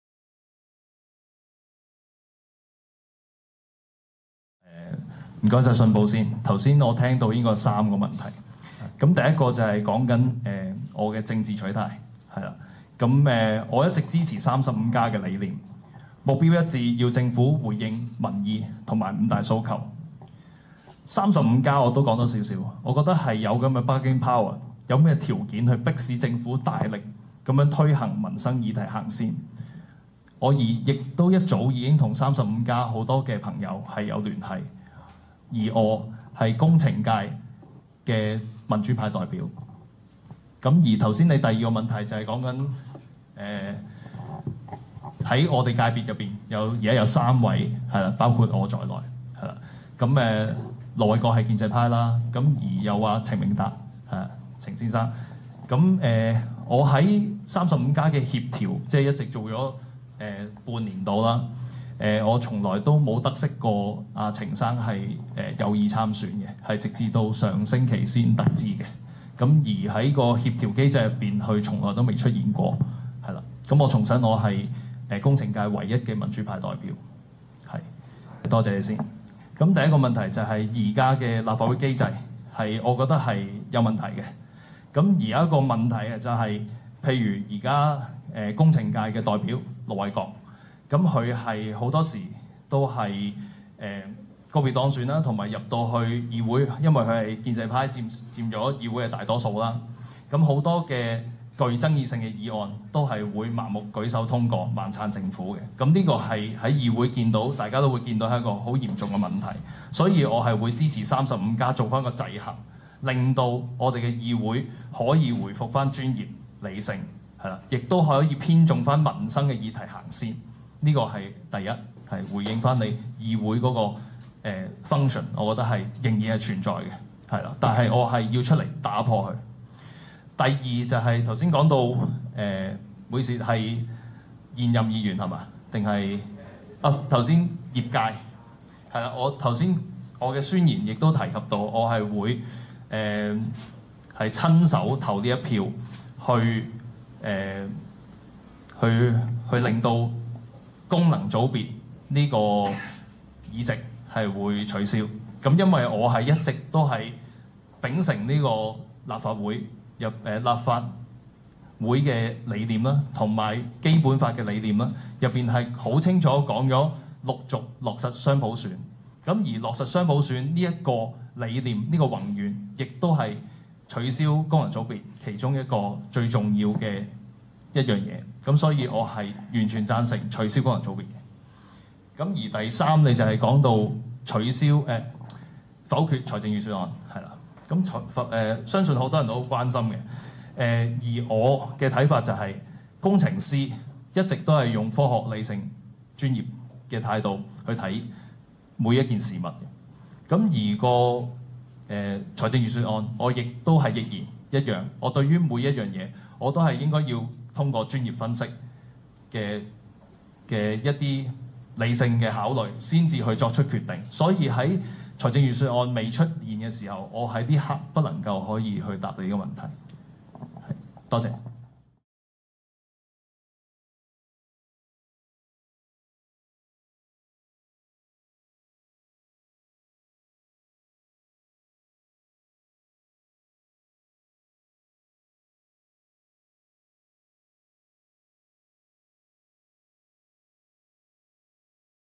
工程師黃偉信宣佈參選立會工程界別選舉，自認是工程界唯一民主派代表，支持雙普選，如果當選，將親自投下「取消功能組別」一票
工程師黃偉信宣布參選立會工程界別選舉，自認是工程界唯一民主派代表，支持雙普選，如果當選，將親自投下「取消功能組別」一票。

中共強推國安法的結局如何🤔？香港成了中美二龍奪珠的主戰場🥊，勝負將如何⚔️？！